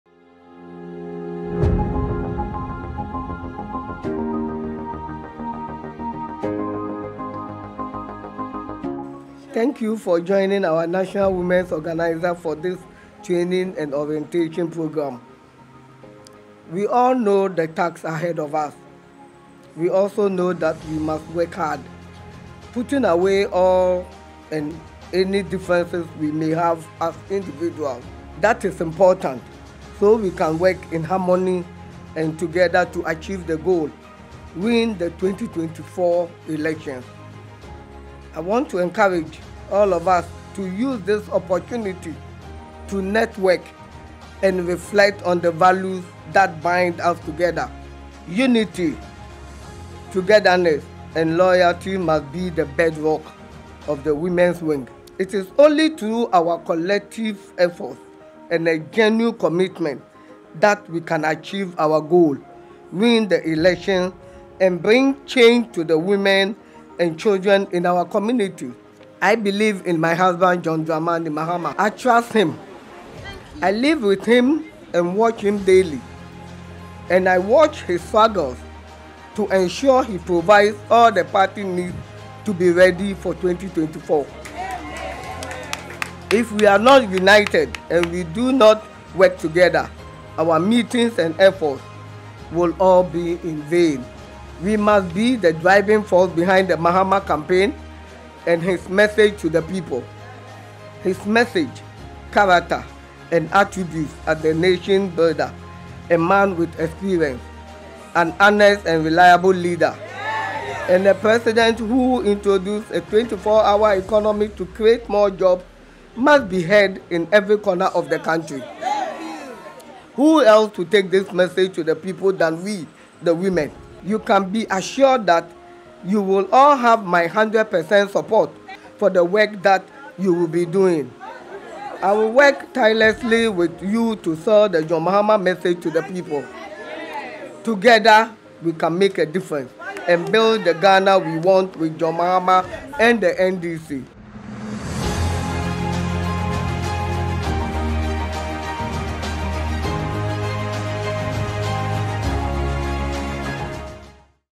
"I believe in my husband, John Mahama. I trust him", says Lordina Dramani Mahama.
We must be the driving force behind the Mahama campaign and his message to the people. His message, character, and attributes as the Nation Builder, a man with experience, an honest and reliable leader, and a president who would introduce a 24-hour economy to create more jobs must be heard in every corner of the country.